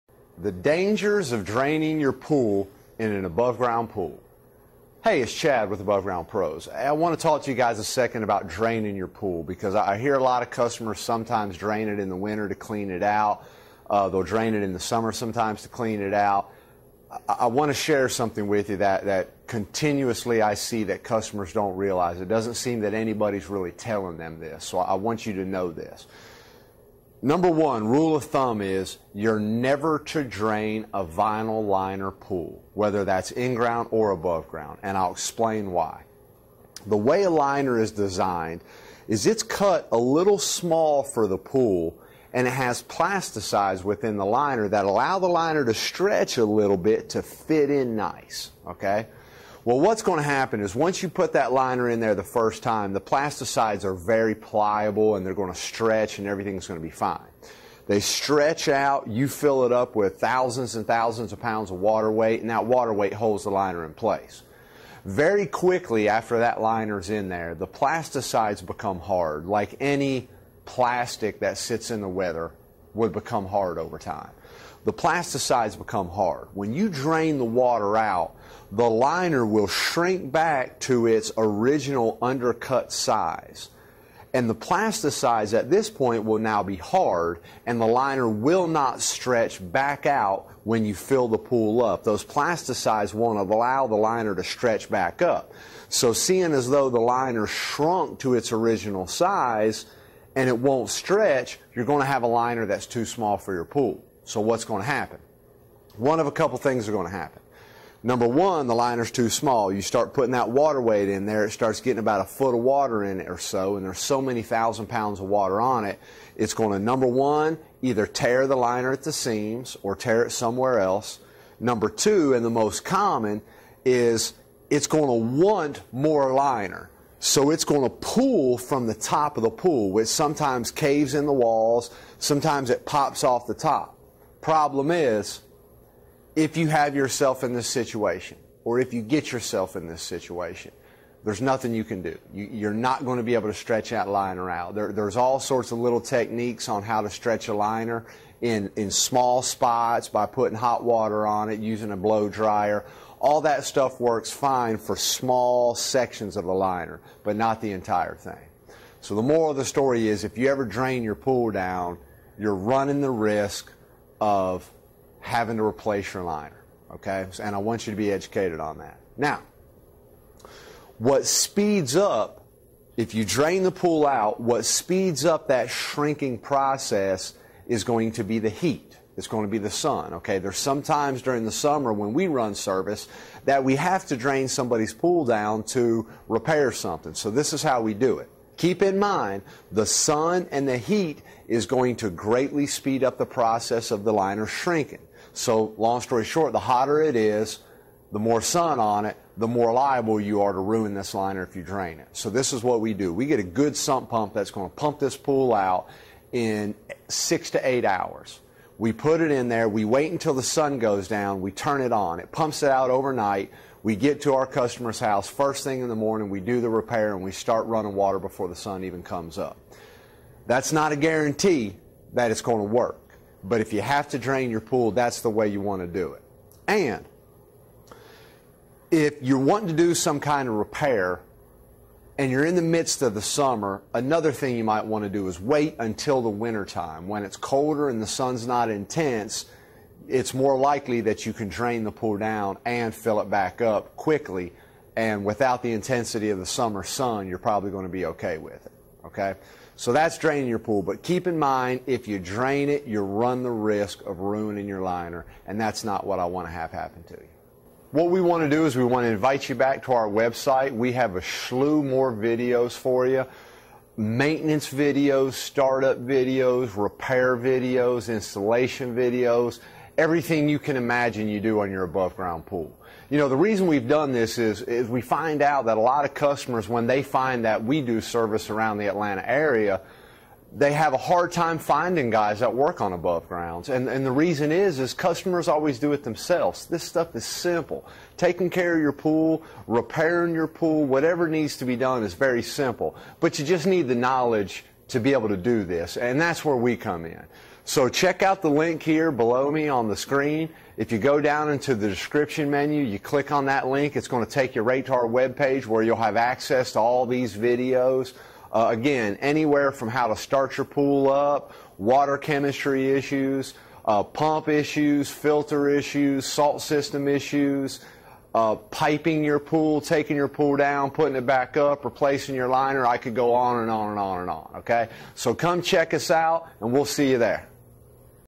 Dangers of Draining your Liner in your Above Ground Pool
Draining your liner can cause some issues.  Watch this video to educate yourself on the dangers of draining your pool.